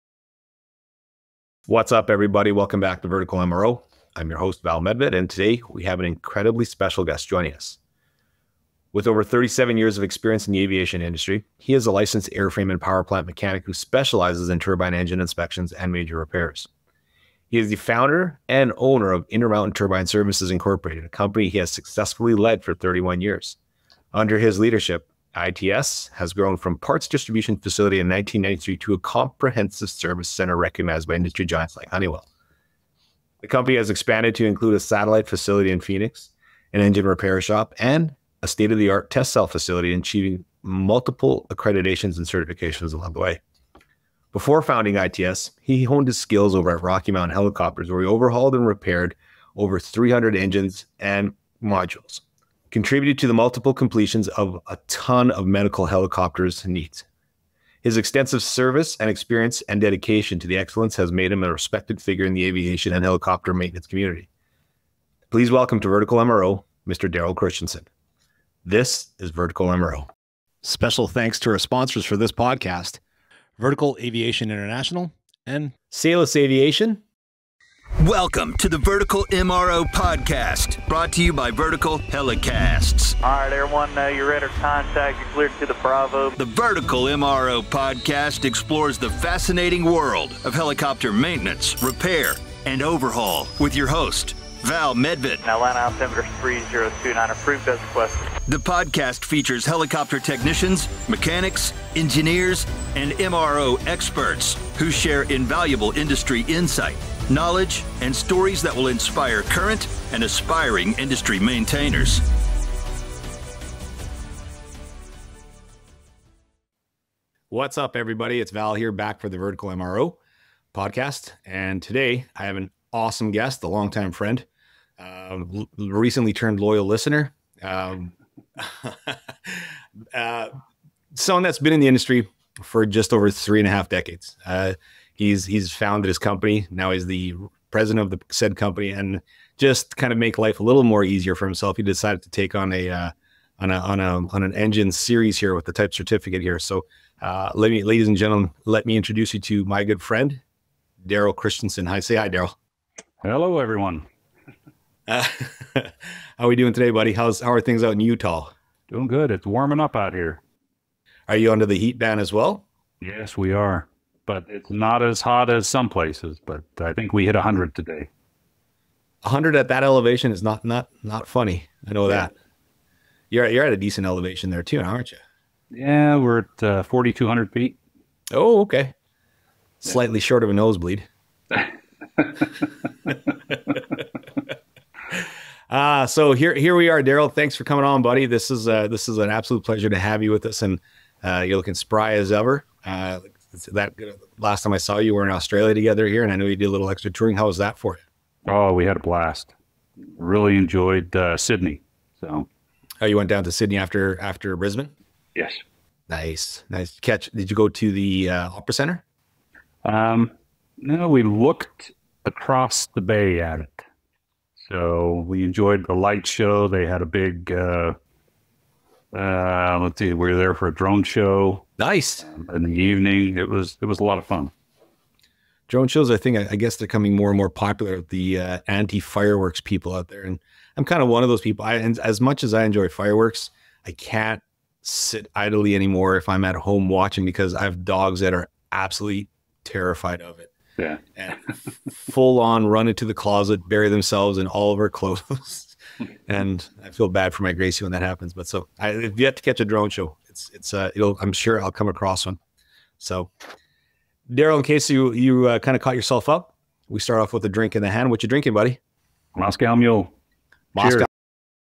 Vertical MRO - Ep 15 - Darryl Christensen, A&P, founder & owner Intermountain Turbine Services Inc.
In today’s episode, we welcome Darryl Christensen to the show.

Darryl is a licensed airframe and powerplant mechanic with over 37 years of experience, specializing in turbine engine inspections and major repairs.

He is also founder and owner of Intermountain Turbine Services Inc., a company he has successfully led for 31 years.

Under Darryl’s leadership, ITS has grown from a parts distribution facility to a comprehensive service center recognized by industry giants like Honeywell.

The company has expanded to include a satellite facility in Phoenix, an engine repair shop and a state-of-the-art test cell facility, achieving multiple accreditations and certifications along the way.

Before founding ITS, Darryl honed his skills at Rocky Mountain Helicopters. His extensive service, experience, and dedication to excellence has made him a respected figure in the aviation and helicopter maintenance community.

Join us for this engaging conversation with one of the MRO industry’s top talents!